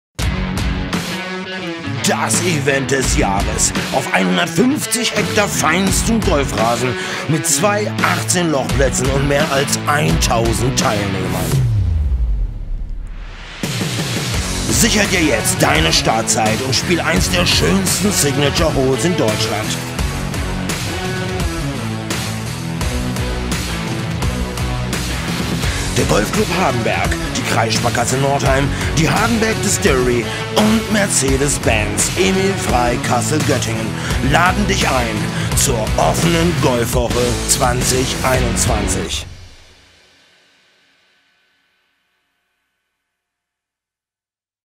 Offene Golf Woche 2021 - Golf Club Hardenberg
Das Event des Jahres auf 150 Hektar feinstem Golfrasen begrüßen wir über 1000 Teilnehmer auf unserem schönen GolfResort im Herzen Deutschlands.